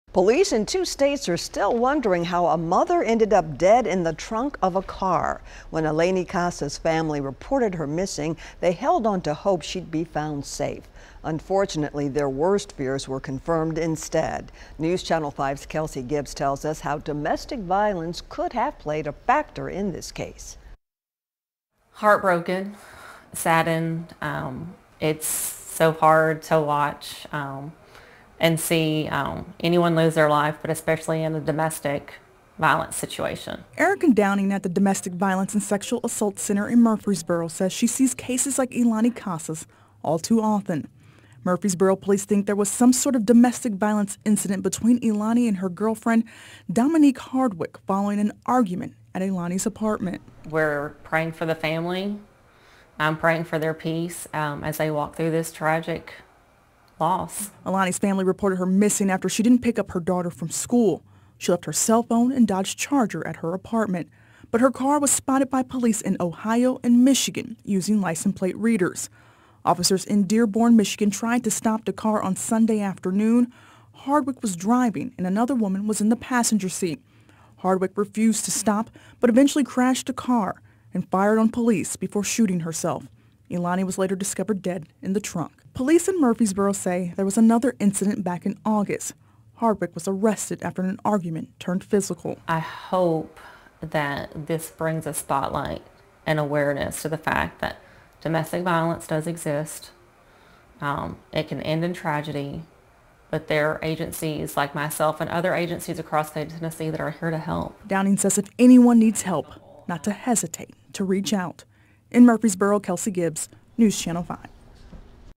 Tennessee Woman tragically found in trunk in Michigan
A missing mother was found dead in a trunk more than 500 miles from home. Eleni Kassa was reported missing from Murfreesboro on November 18. Police believe she was the victim of domestic violence.